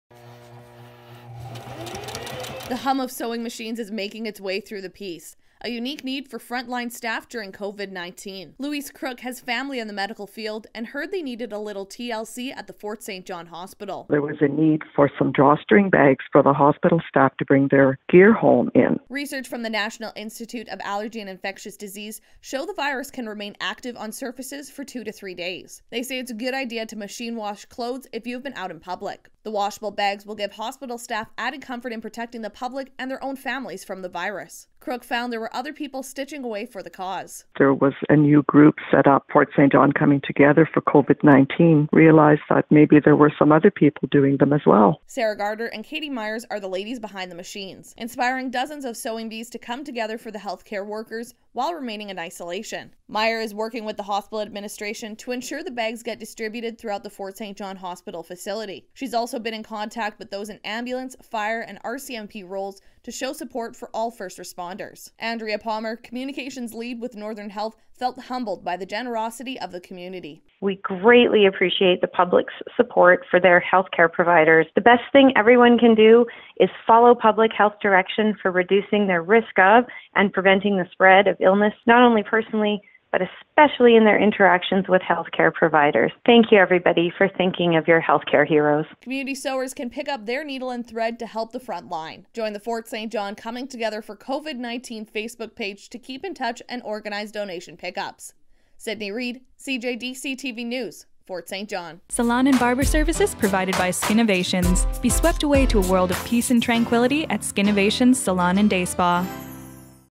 Washable Bags for Hospital Staff
Looking to pick up a new hobby, or maybe dust off an old one?
CJDC's Sydney Reid reports on a local initiative to make washable bags for the staff at the Fort St. John Hospital.